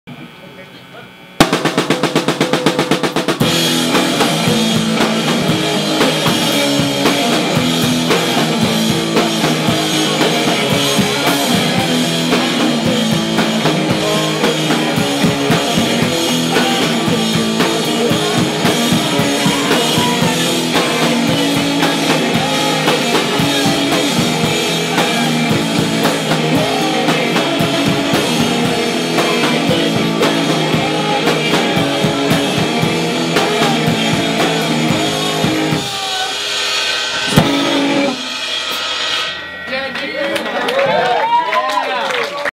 Stinky Smelly - song 6
Stinky Smelly performing during night 4 of Kat Kat Records Phest Two on December 31st, at The Fire in Philadelphia, Pa.